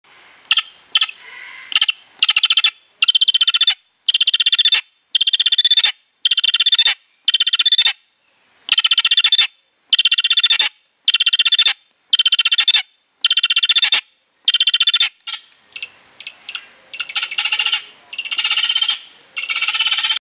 vulturine guineafowl sings
very short clip of a singing Acryllium vulturinum
avifauna alphen aan de rijn, holland